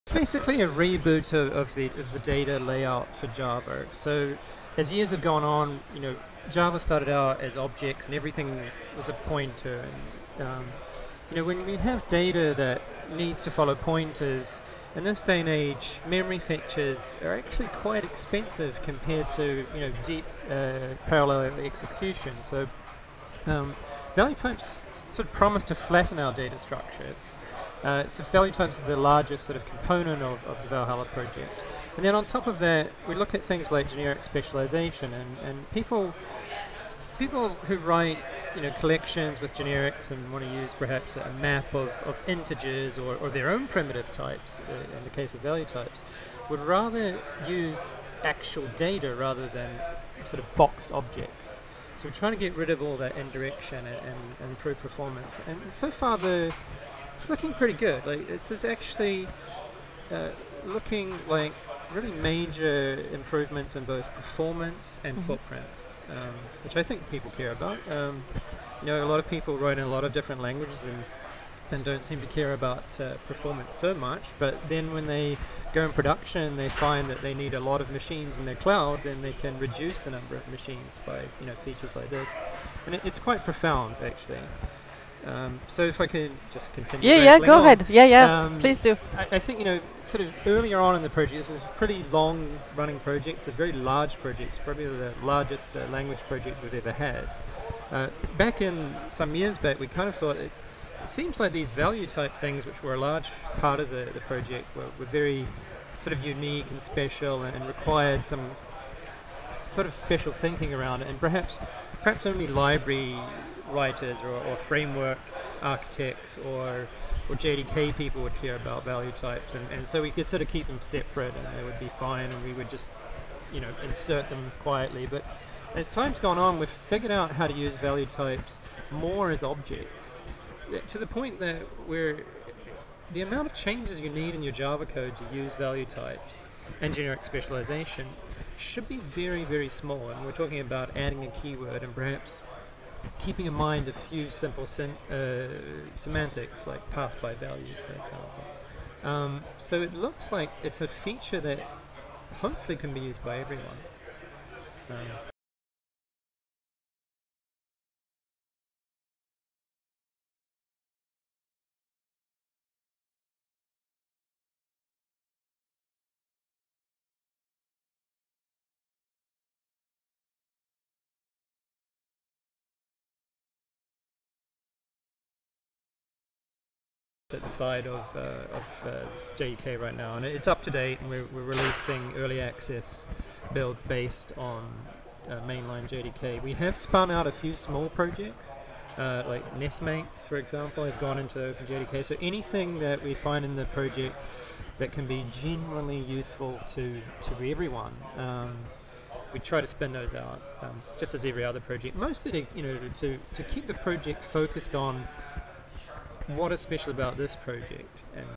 Project Valhalla, Path to Value Types in the JDK
A presentation on the current status of Project Valhalla. After releasing two prototype early access builds, "Minimal Value Types" and "L-World (LW1)", the project is adding support for value types in generic erasure, and devised a plan for inter-operation with new value types and legacy code.

There will be a brief overview of Value Types, what they are, why and when to use them, including some code examples highlighting their identity-less, immutable and non-null characteristics. This includes a look at the concept of "data flattening" and it's impact on the programming model.